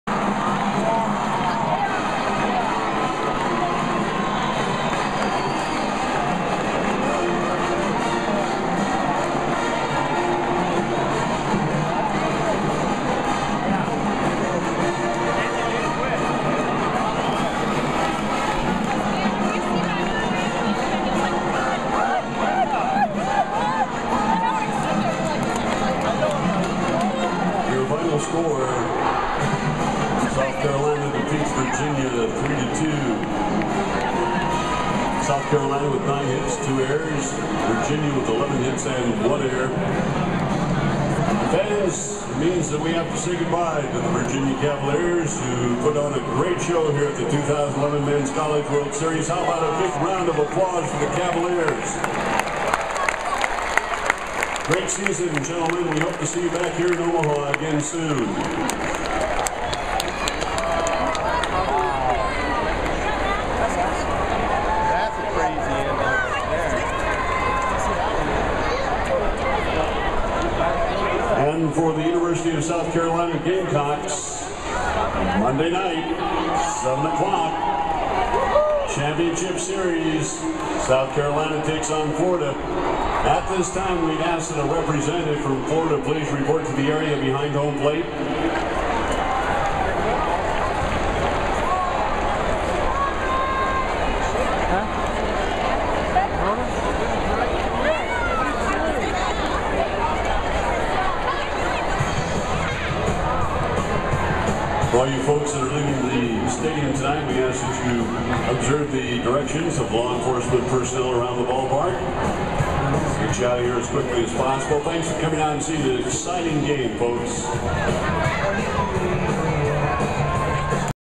College World Series Game, SC vs UVA, June 24, 2011
This was after SC scored the final run to advance to the CWS championship against Florida.